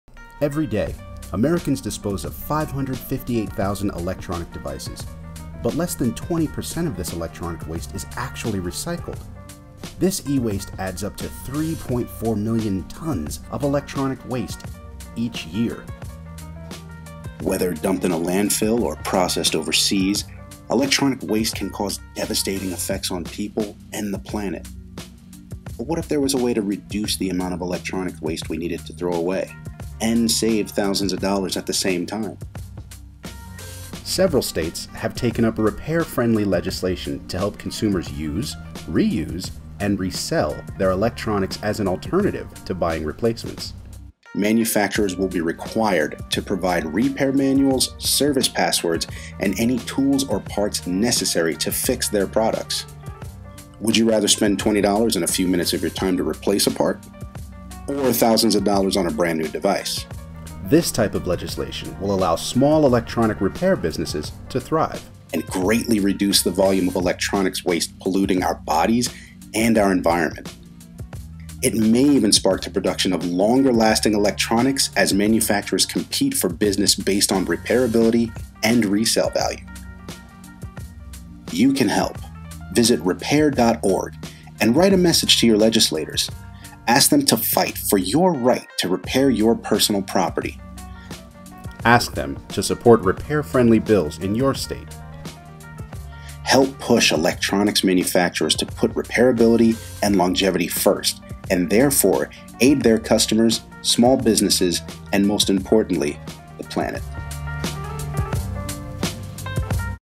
Digital Right To Repair - Repair.org
Voiceover: Vince Oliver

Americans dispose of 558,000 electronic devices EVERY DAY, and less than 20% of these are properly recycled. One reason for this is that our electronics can be difficult or even impossible to repair. Repair.org is working to enact legislation that will require major electronics manufacturers to provide manuals, passwords, and tools necessary to repair their products. This will allow small businesses to endure and grow, and prevent these large corporations from driving small businesses out of the marketplace. It will also help to maintain your right to repair your personal property.